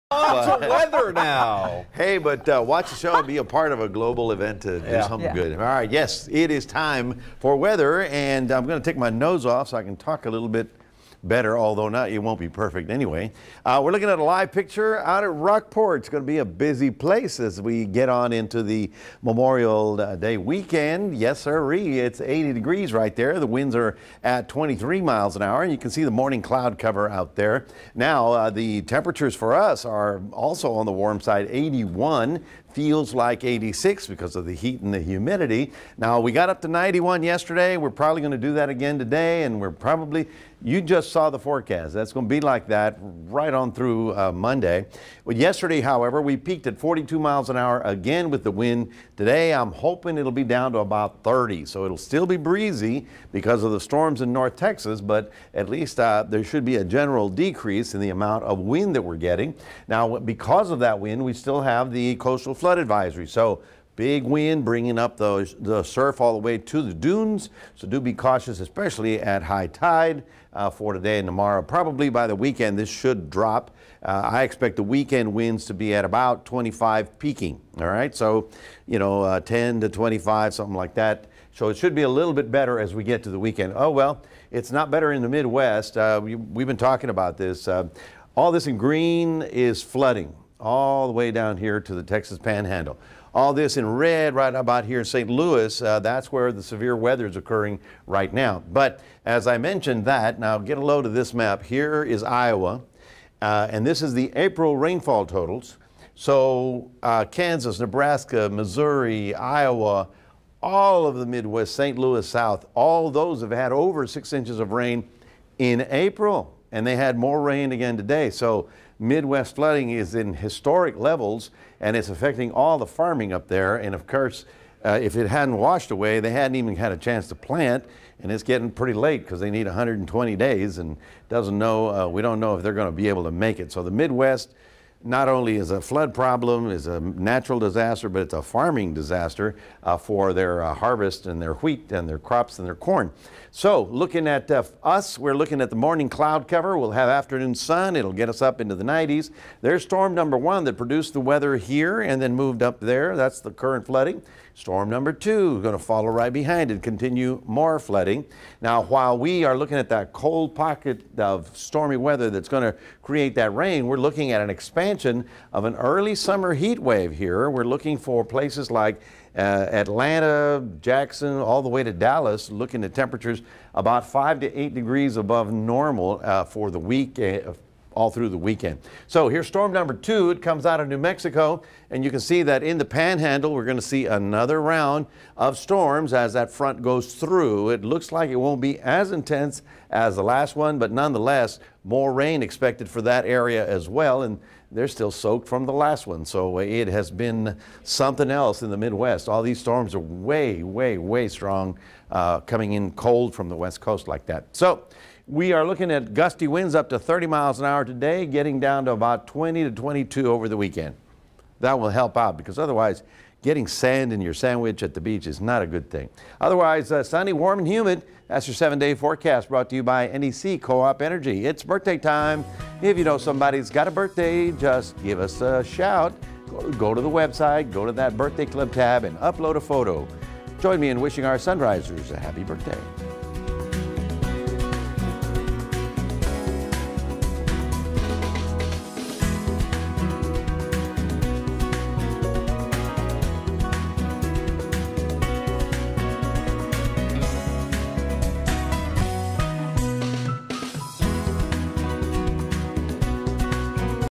Expect sweltering humidity into weekend
Strong south winds and strong high pressure will keep us on the hot and steamy side of things.